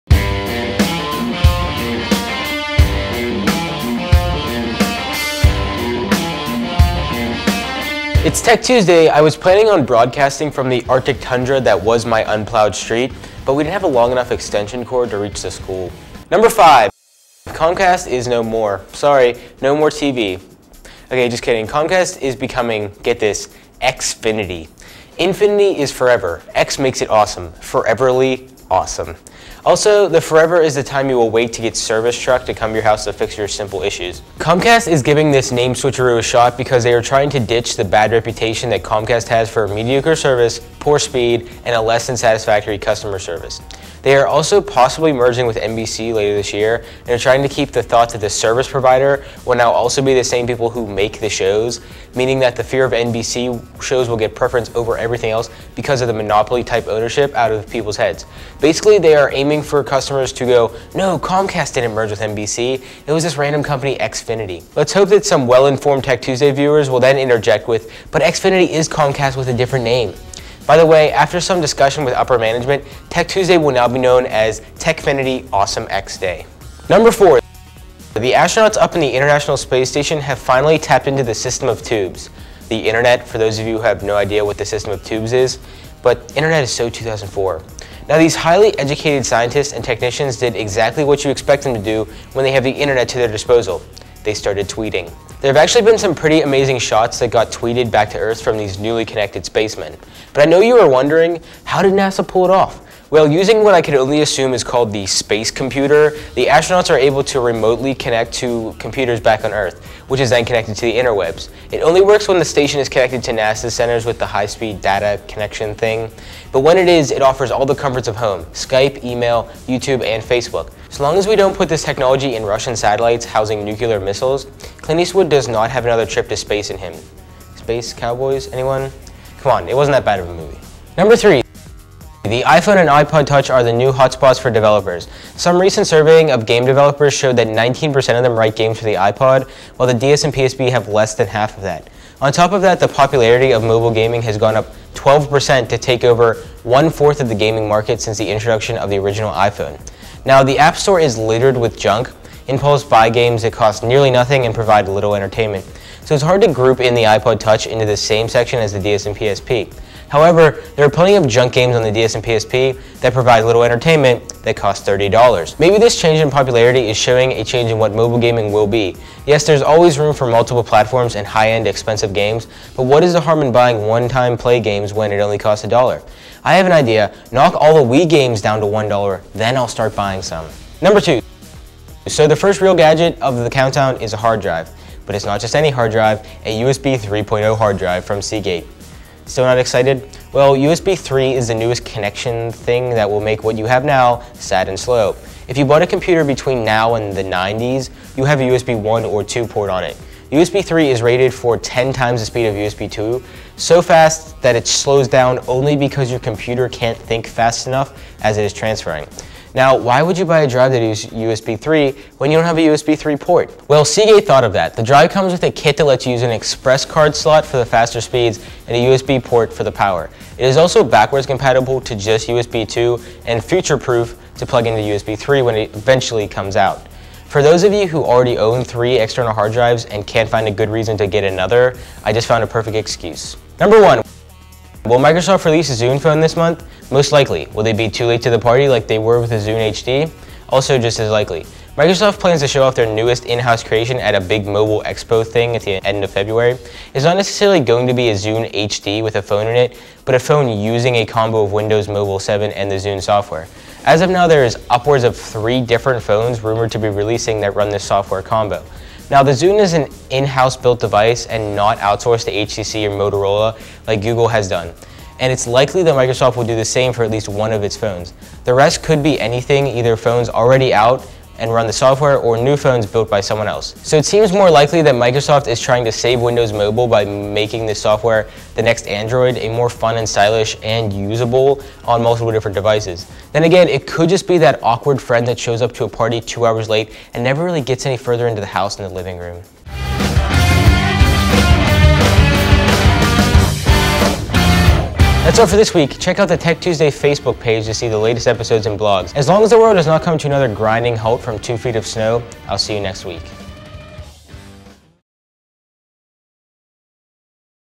Tech Tuesday 2-16
Back on the air after a snowpocalypse with comcast trickery, Space internet, iphone gaming, the newest hard drives that you cant use, and early reports on the new Zune Phone. Also humor, sorta- thats more a matter of opinion